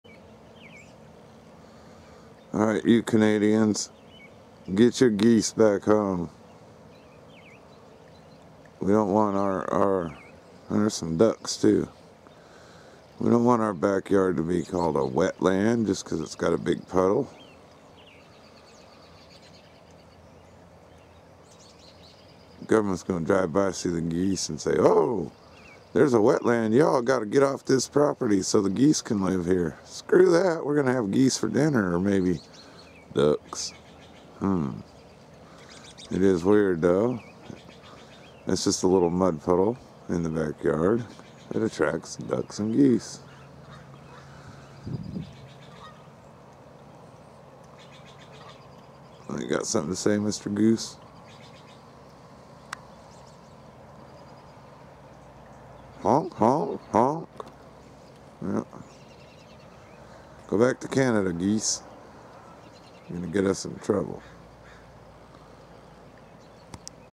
Canadastan Geese causing me a headache!
We have had nothing but rain and a creek right by this puddle is overflowing from snowmelt-Now the UN-(Agenda21) wants to say people who have puddles are wetlands-Mind you this summer it will be bone dry